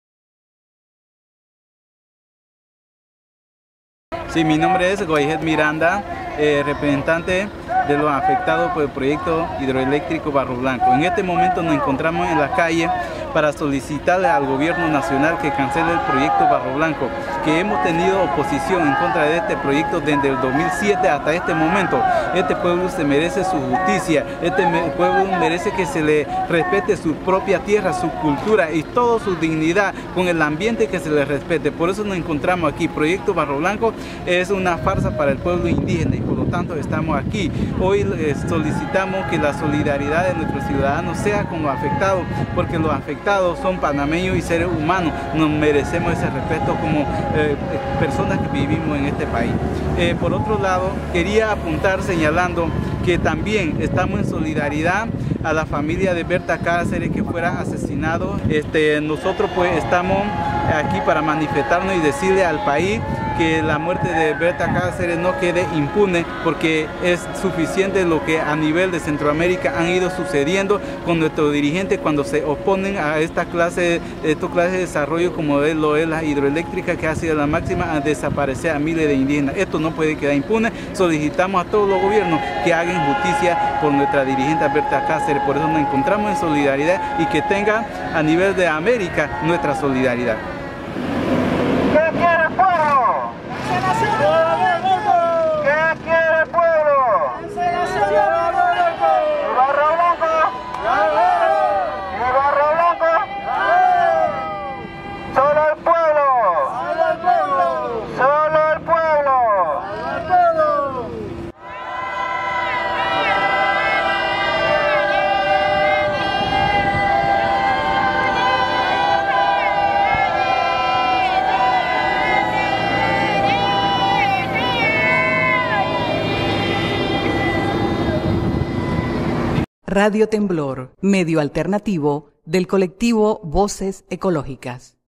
Protesta indígena Ngäbé Büglé en rechazo a la hidroeléctrica Barro Blanco
Indígenas Ngäbe Büglé se manifestaron el miércoles 15 de junio, sobre la vía interamericana, en rechazo a la hidroeléctrica Barro Blanco que represa el río Tabasará.

Cuya unidad y acción indígena, tuvo como objetivo denunciar las violaciones a los Derechos Humanos Indígenas y Territorial, que el proyecto energético a generado desde su concesión hasta el inicio de pruebas de llenado, que no estaba contemplado en los acuerdos entre la dirigencia indígena y el Gobierno Nacional, dirigido por el presidente Juan Carlos Varela. Acto que se adiciona, a las irregularidades e incumplimientos las normas administrativas, ambientales y de los Derechos de los Indígenas.

En esta protesta, se presento el Defensor del Pueblo, Alfredo Castillero, junto a su personal técnico, en donde extendió su colaboración con los afectados, los procedimientos adecuados para lograr resultados positivos para la continuidad del desarrollo del pueblo Ngäbé y la no violación a sus Derechos Humanos.

En el marco de esta manifestación, la dirigencia indígena, bajo su costumbre y mística tradicional y espiritual Mama Tatda, expreso su solidaridad con los pueblos de Latinoamérica oprimidos por estos proyectos extractivos, enfatizando la solidaridad con el pueblo de Honduras, y la Acción Global de justicia ante el asesinato de la indígena lenca Berta Cáceres.